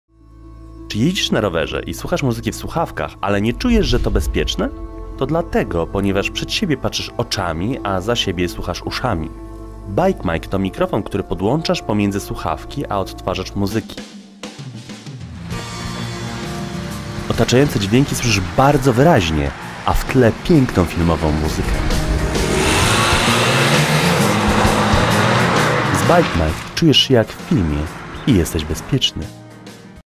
BikeMic - Short presentation.
Coraz więcej osób podczas jazdy na rowerze słucha muzyki w słuchawkach, odcinając się w ten sposób od dźwięków otoczenia. Jest to zachowanie bardzo dla nich niebezpieczne. BikeMic to specjalny mikrofon podłączany pomiędzy odtwarzacz mp3 (np. telefon) a słuchawki, który do odtwarzanej muzyki dokłada dźwięki z otoczenia. Z BikeMic słyszymy doskonałej jakości muzykę oraz otoczenie i możemy niezależnie regulować głośność obu tych dźwięków. Z BikeMic muzyka na rowerze staje się bezpieczna.